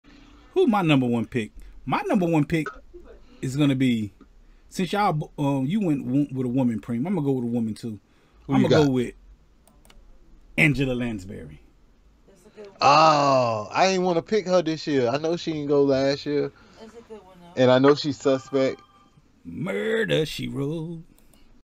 Angela Lansbury DEAD at 96 😥 Murder She Wrote
As I pick in the 2022 Talk Dat Tawwwk Celebrity Dead Pool Draft, Actress Angela Lansbury has passed away at the age of 96 R.I.P Angela Lansbury. 😥
Maaaaaaan!!!